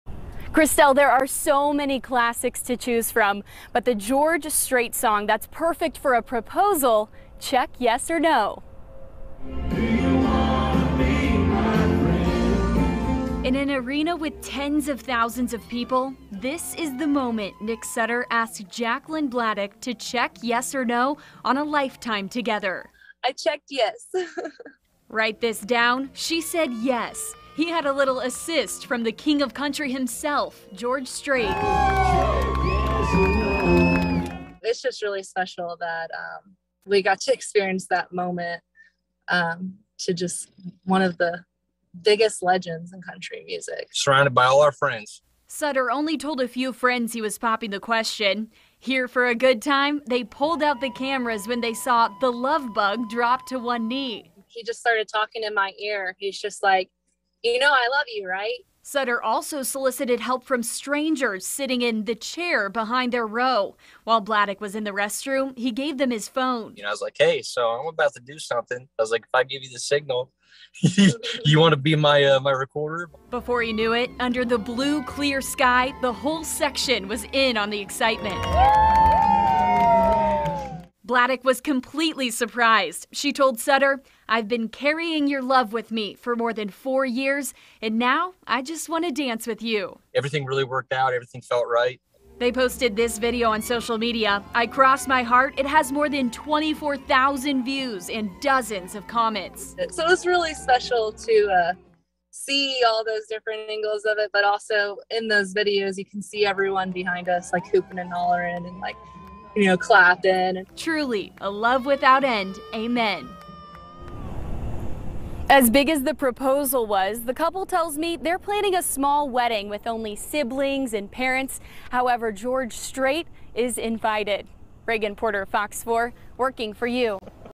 Missouri couple gets engaged at George Strait concert
Country wedding bells are ringing for one Missouri couple. They got engaged at the George Strait concert over the weekend. The King of Country performed at Arrowhead Stadium.